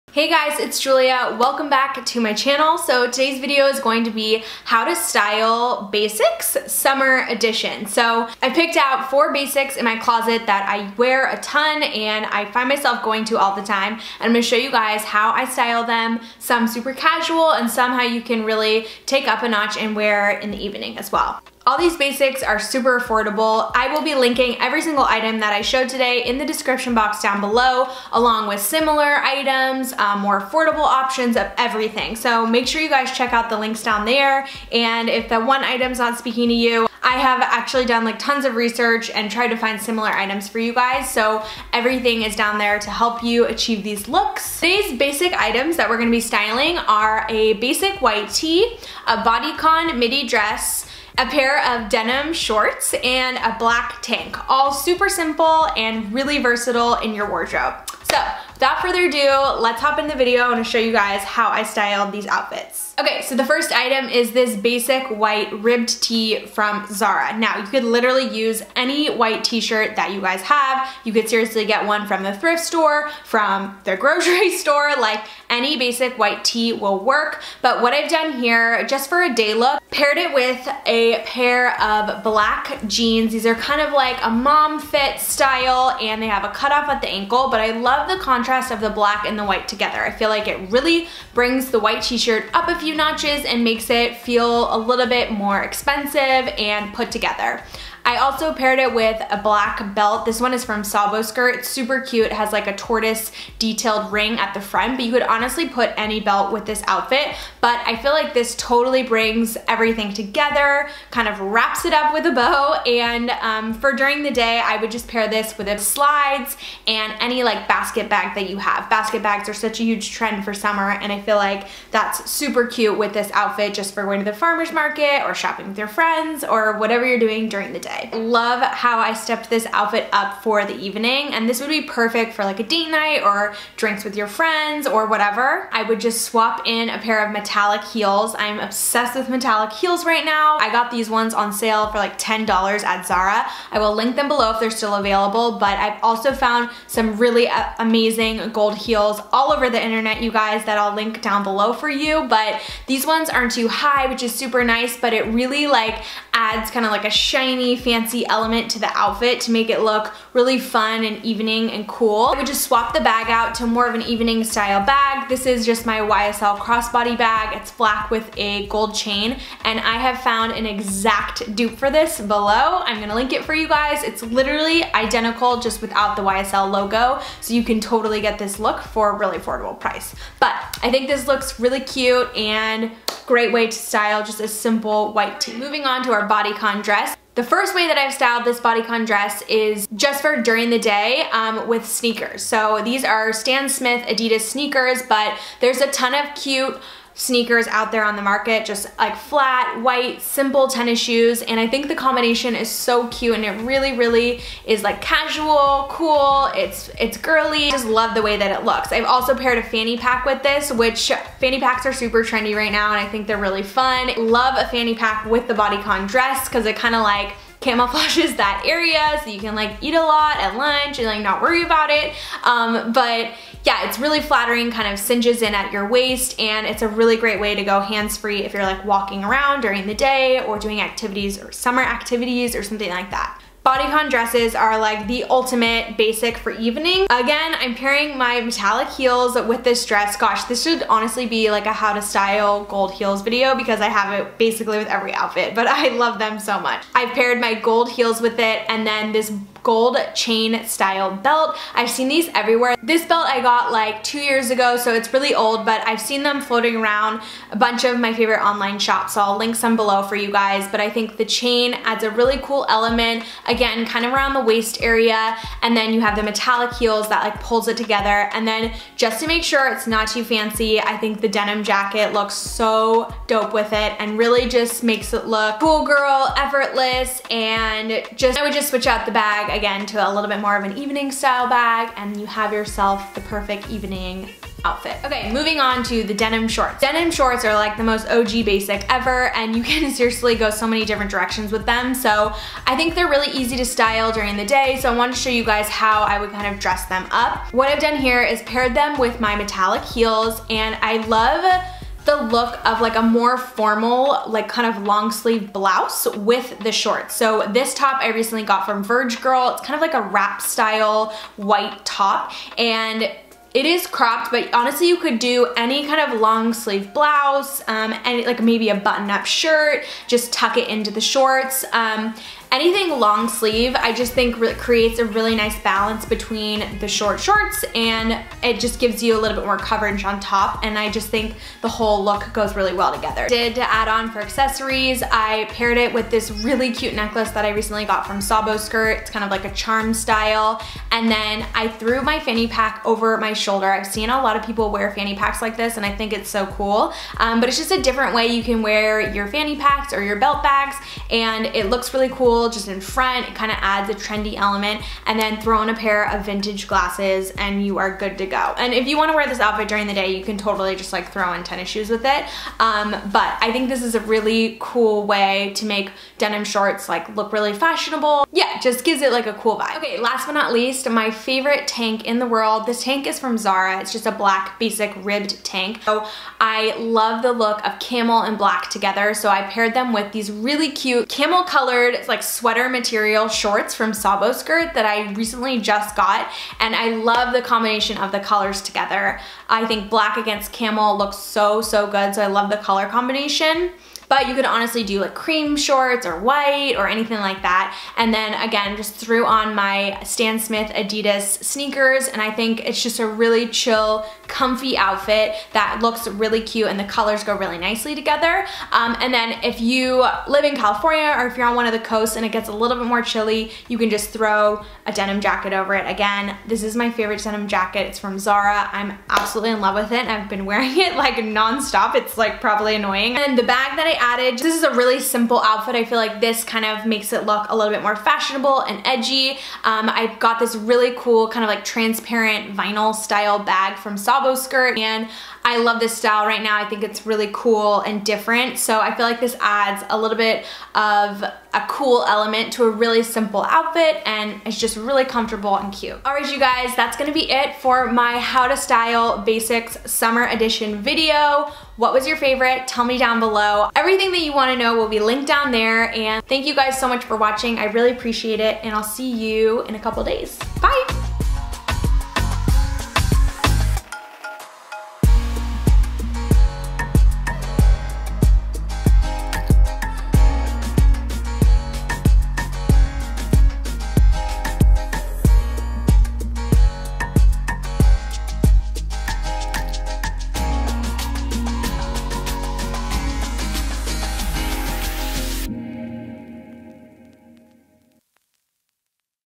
HOW TO STYLE BASICS! SUMMER | Julia Havens
Today's video I'm showing you guys how to style  basics summer edition! I have chosen some of my favorite basics from my closet and showing you guys how I would style each piece during the day and into the evening. We are going to walk through how to style these basics: white tee, bodycon midi ress, denim shorts, and a black tank. I have soooo many dupes and more affordable items linked for you guys down below as well! Thumbs up for more styling videos!

OUTFIT DETAILS!

Black Jeans (Vintage Cropped fit from H&M- old)
similar

BLACK TANK

I N S T A G R A M (@jujhavens)